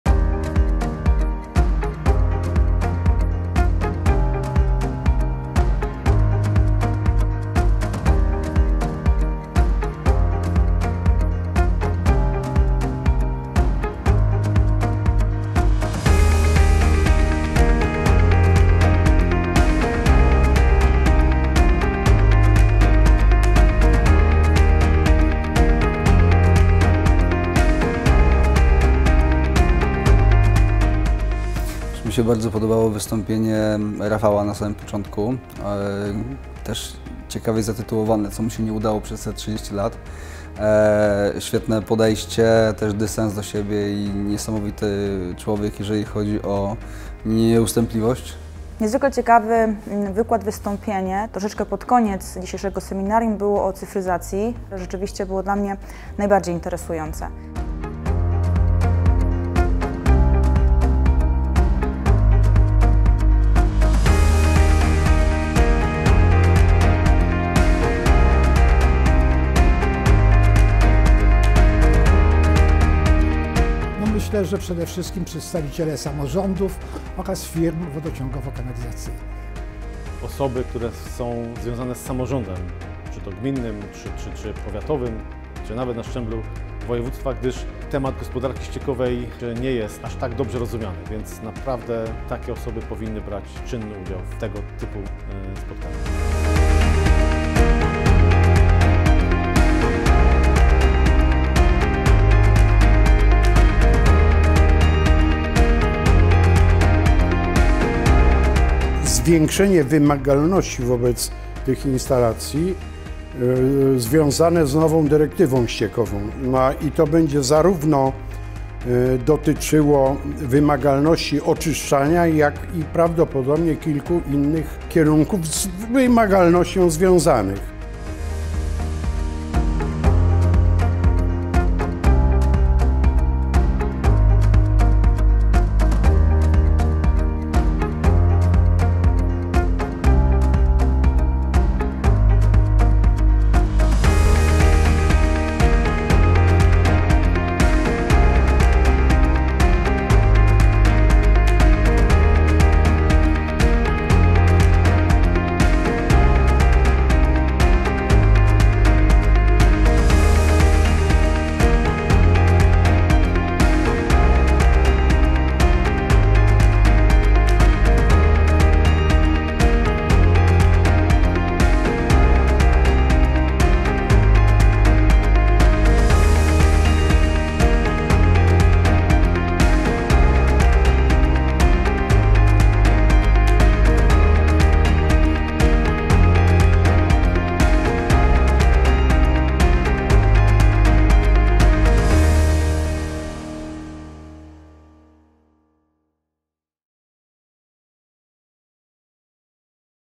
30 lat Haba Sympozjum Naukowe - film jubileuszowy
Zapraszamy do obejrzenia, krótkiej relacji z tego rocznego sympozjum naukowego.  Tym razem wydarzenie miało dla nas jeszcze bardziej wyjątkowy charakter.  Zaskakująca frekwencja, ale również 30 lat funkcjonowania naszej firmy.  Dziękujemy wszystkim uczestnikom, za przybycie i możliwość połączenia wiedzy świata nauki oraz doświadczenia naszych przedstawicieli i współpracujących.

Po więcej informacji zapraszamy do bezpośredniego kontaktu z biurem obsługi klienta.

Masz pytania?